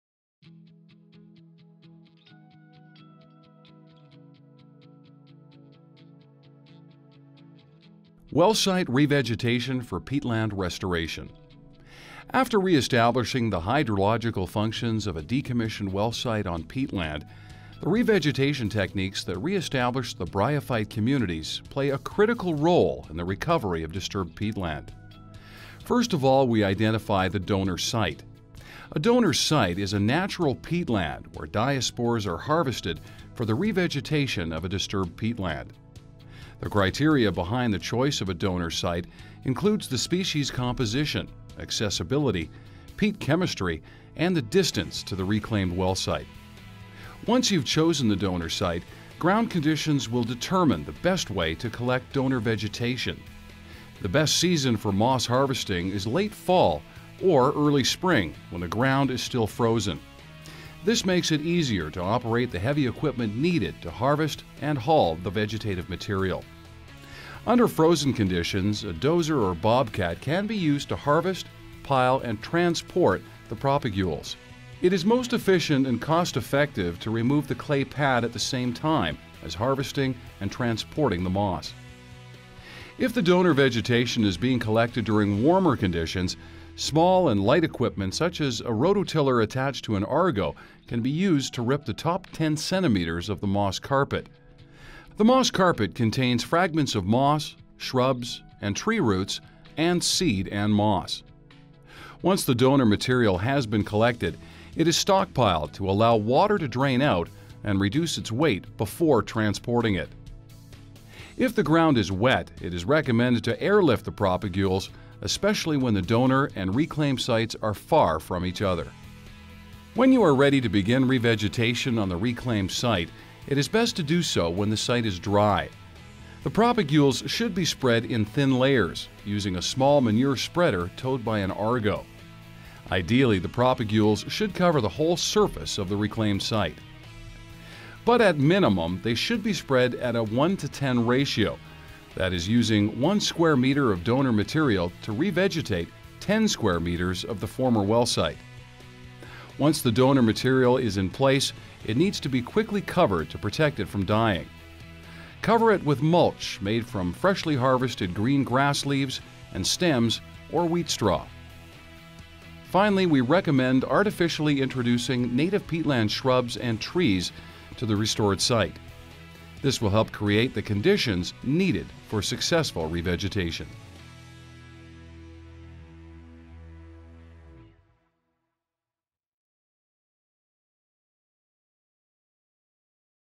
NAIT Boreal Research Institute Peatland Restoration
This video describes how NAIT's Boreal Research Institute in Peace River introduces appropriate pioneer species to facilitate the re-vegetation process on peatland.

Site re-vegetation is an important phase in peatland restoration. Prior to re-vegetating a peatland site, appropriate methods should be used to adjust the soil and address the hydrologic disturbance. At NAIT Boreal Research Institute (NBRI) we have successfully implemented the Peat Inversion technique to connect water flow between the surrounding natural peatland and the reclaimed site, thus facilitating the establishment and development of vegetation.